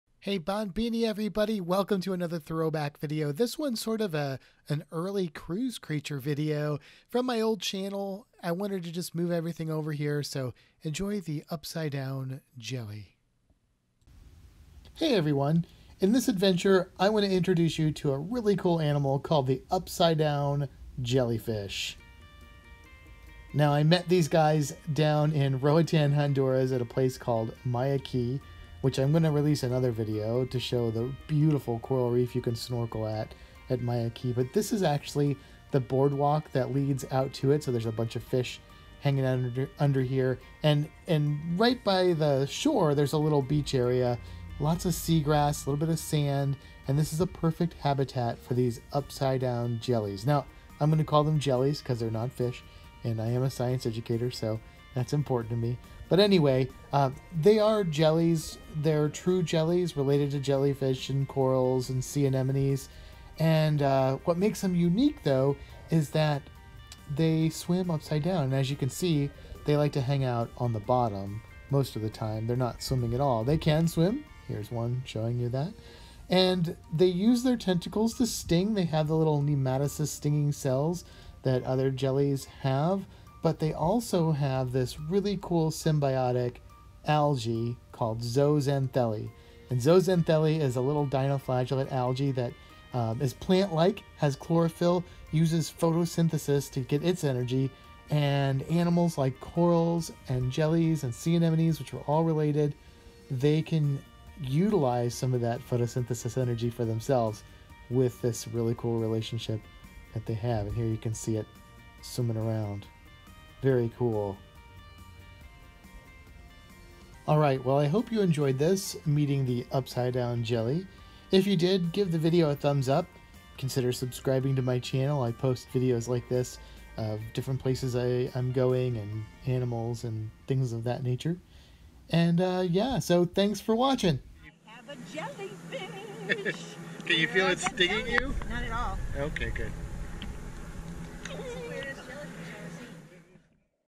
upside down jellyfish
In this throwback video we meet the amazing upside down jellyfish.